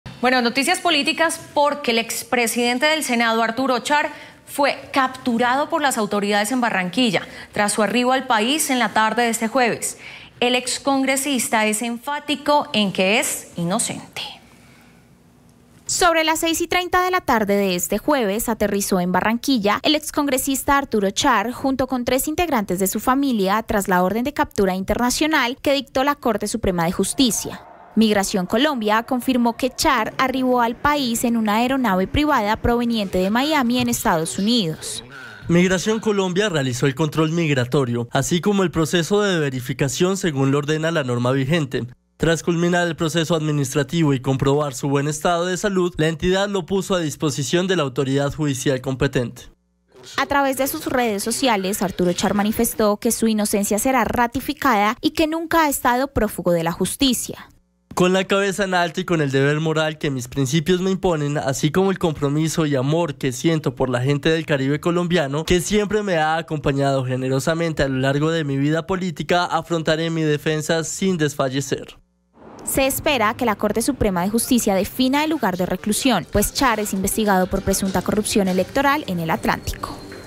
Arturo Char fue capturado en Barranquilla tras llegar al país
"Nunca he estado prófugo de la justicia": exsenador, Arturo Char, tras viajar a Colombia y ser capturado por las autoridades en Barranquilla